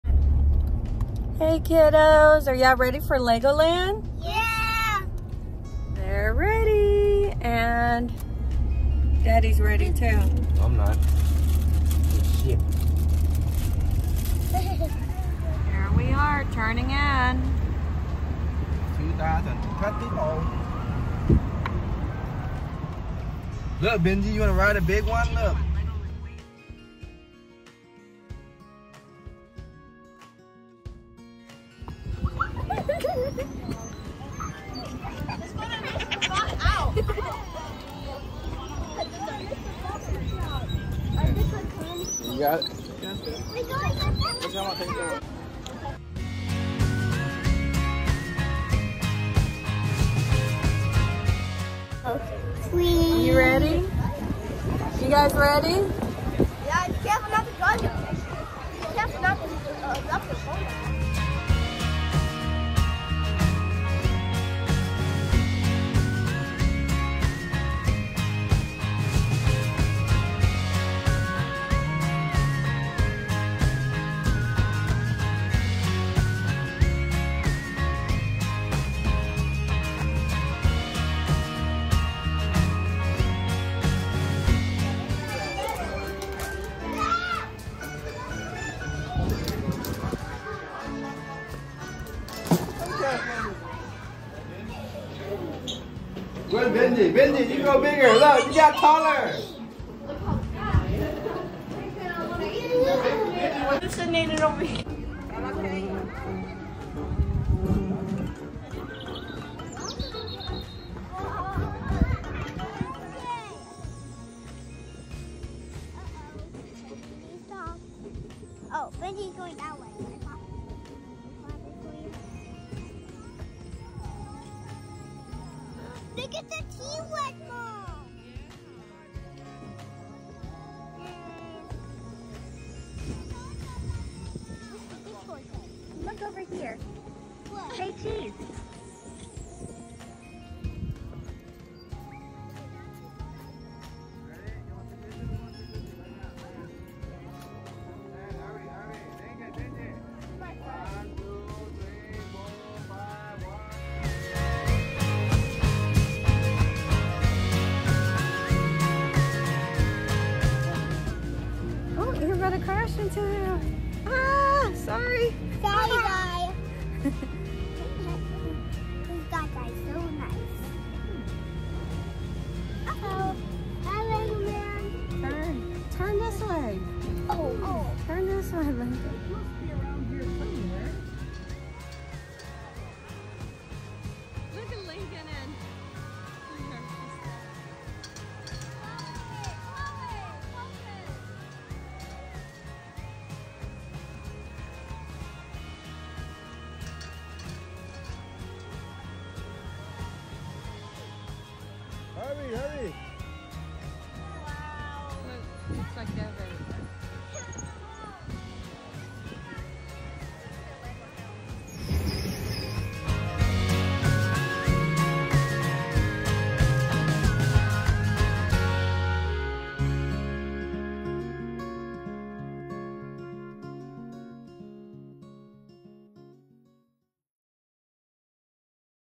Life Vlog with Kiddos for Winter Break 2024
Quick getaway with the party of 5 to Orlando, Florida. Kids wanted Legoland but we surprised the younger kids with Peppa Pig park as well. Love them at this age. Documenting a day in our life. Enjoy! :)